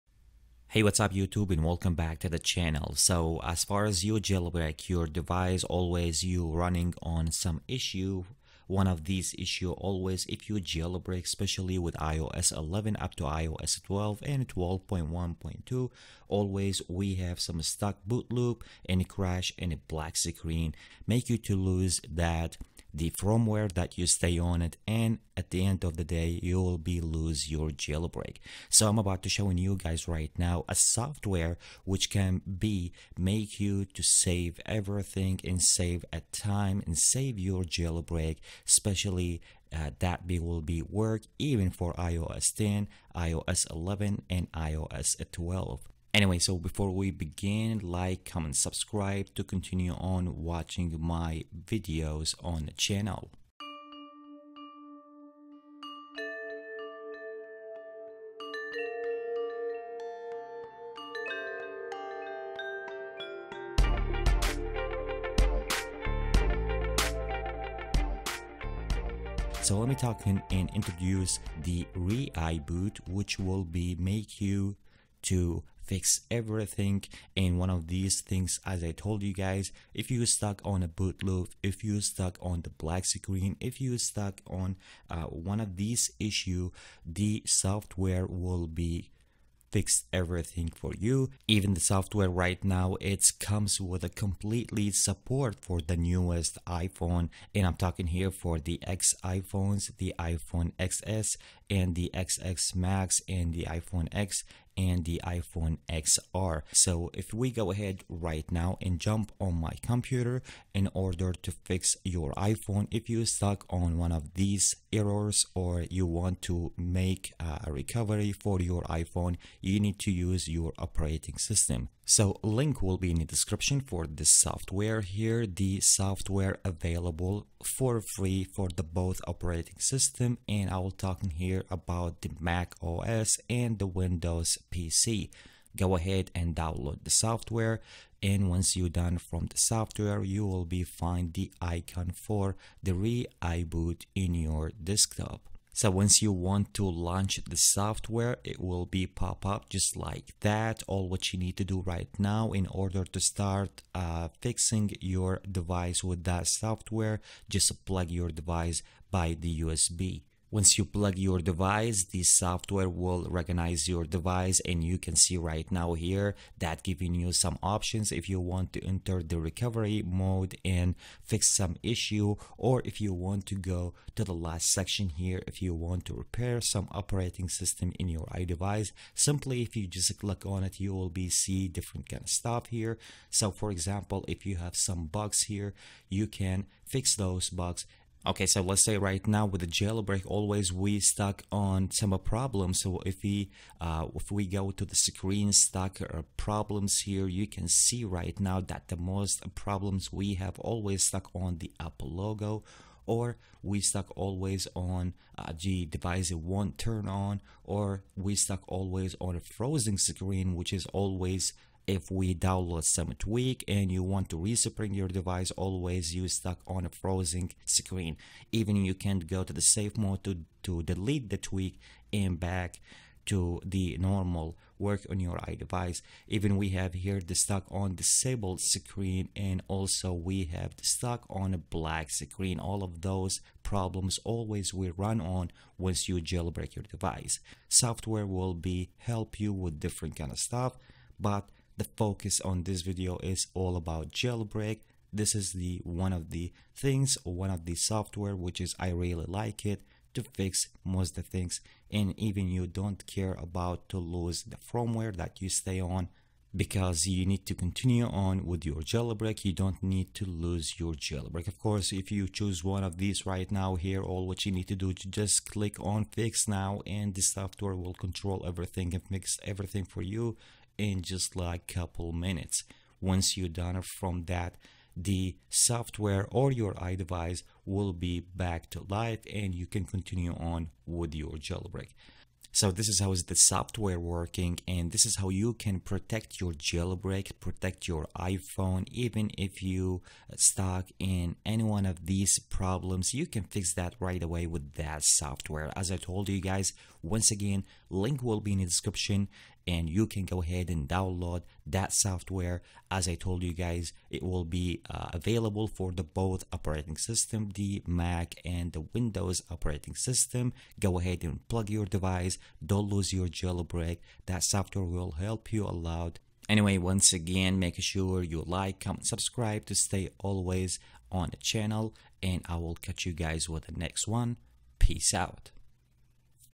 How To Fix Stuck BootLoop /Black Screen/ Recpring Loop/ Recovery Mode / IOS 11 - 12 With ReiBoot
Fix iPhone BootLoop, Exploit Error, stuck in Apple logo, black screen, keeps crash, or even bricked iPhone after
iOS 11up to ios 12  jailbreak failure. 1 Click Only. No Data